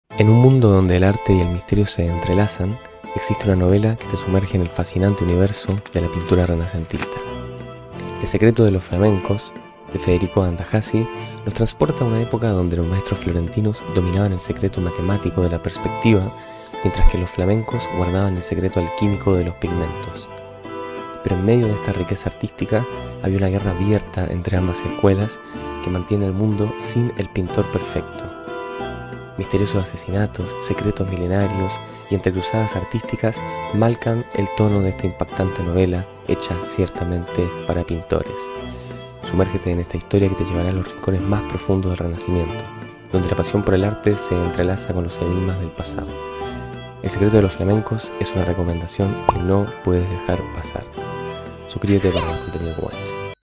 Libro #12: El secreto de los flamencos - Federico Andahazi
A principios del Renacimientos los maestros florentinos dominan el secreto matemático de la perspectiva, y los flamencos el misterio alquímico de los pigmentos. No obstante, una guerra abierta entre amas escuelas mantiene al mundo sin el pintor perfecto, aquel que domingo lo mejor de ambas escuelas.

Misteriosos asesinatos, secretos milenarios y entrecruzadas artísticas marcan el tono de esta impactante novela, hecha ciertamente para pintores. 🐭